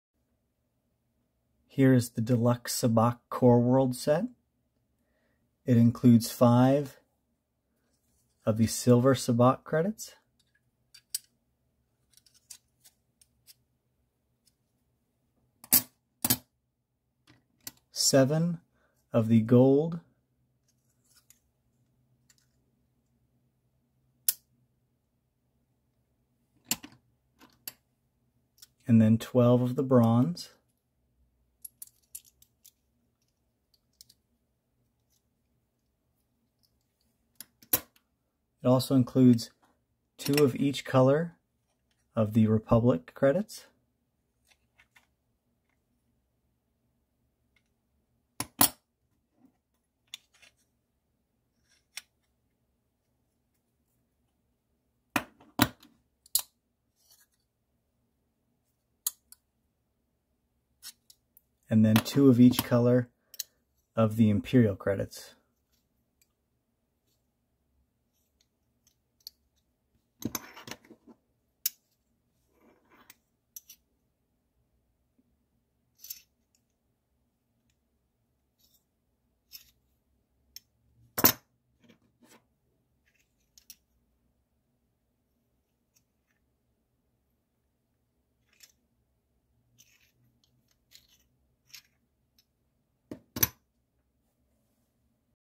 Deluxe Sabacc Core Worlds Set Solid Metal
Even though the Empire is gone, I’ve heard that the credits still spend.

Whether you want to relive the glory days of the Empire or bribe some low-ranking Imperial official, these are the credits for you. These solid metal credits will easily let you pass through any Imperial checkpoints (with or without the correct paperwork).

When the official hears the *clank* of these weighty credits as they hit the table, he won’t ask twice about why you have a shipment of weapons headed to Hoth.

Take your Sabacc games to the next level, check out our supply of *real* Star Wars credits
Get a Monthly Bounty Package, Early Access to products, and more